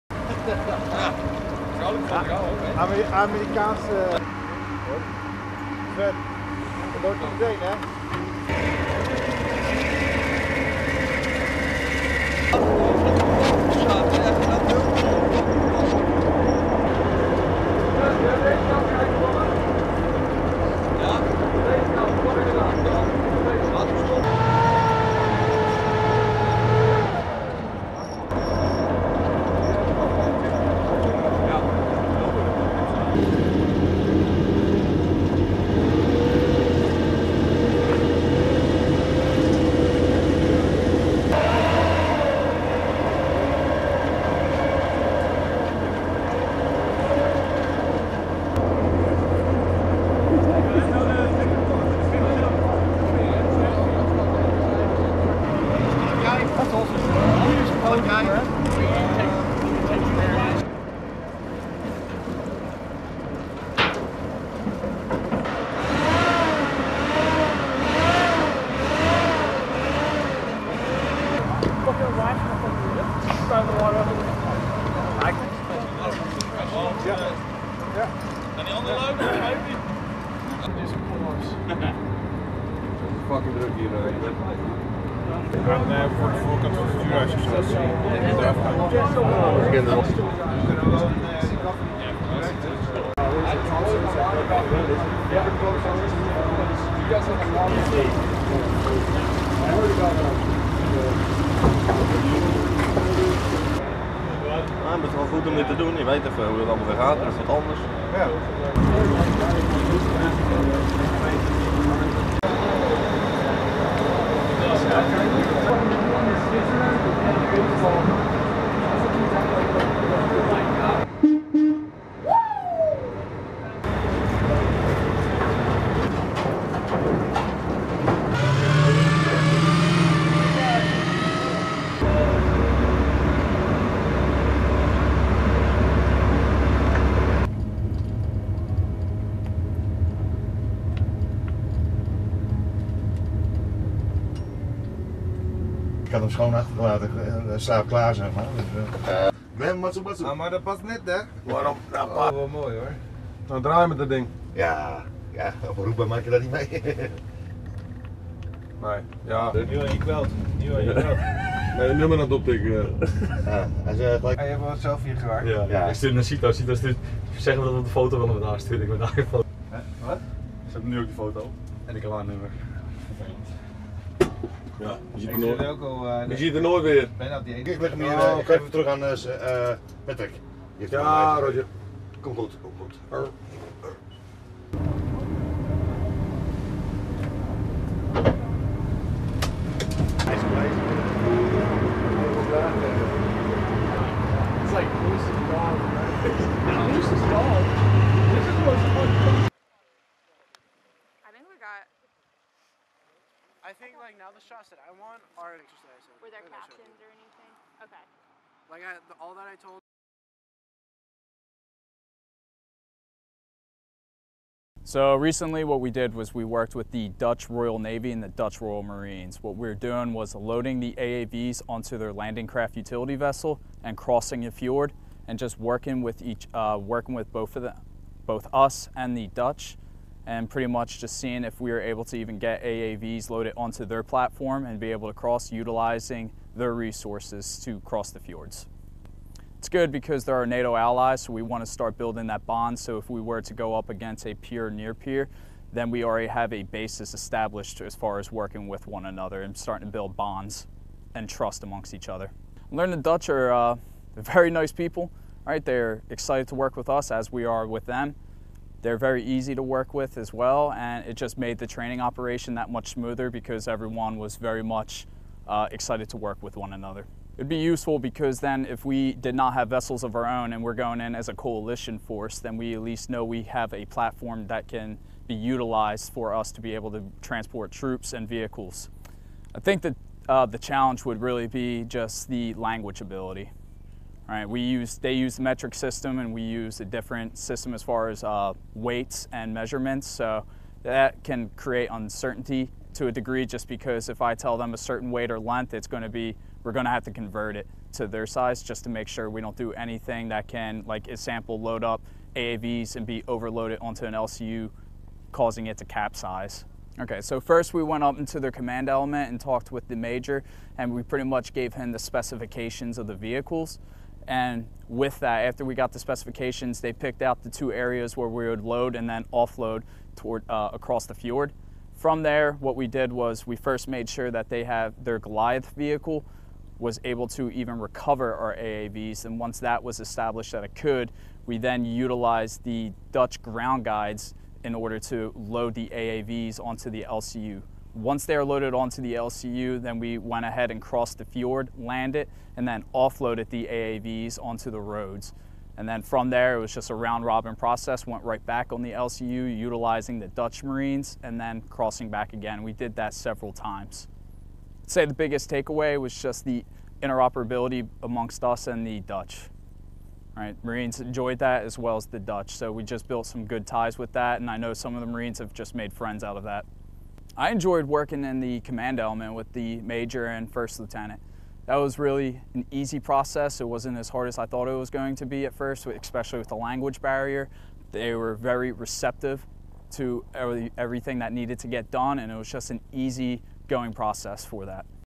"Amphibious Training: U.S. Marines & Dutch Royal Marines at Olavsvern, Norway"
U.S. Marines with Marine Rotational Force Europe 21.1 (MRF-E), Marine Forces Europe and Africa, and Dutch Royal Marines conduct an amphibious training event in Olavsvern, Norway, Feb. 24, 2021.

AAV Training with the Dutch military B-Roll
SETERMOEN, NORWAY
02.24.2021

U.S. Marines and Dutch Royal Marines participated in an integrated training event involving the transportation of assault amphibious vehicles aboard landing craft, utility boats. MRF-E focuses on regional engagements throughout Europe by conducting various exercises, arctic cold-weather and mountain warfare training, and military-to-military engagements, which enhance overall interoperability of the U.S. Marine Corps with allies and partners. (U.S. Marine Corps video by Cpl. Andrew J. Smith)

Film Credits: Video by Cpl. Andrew Smith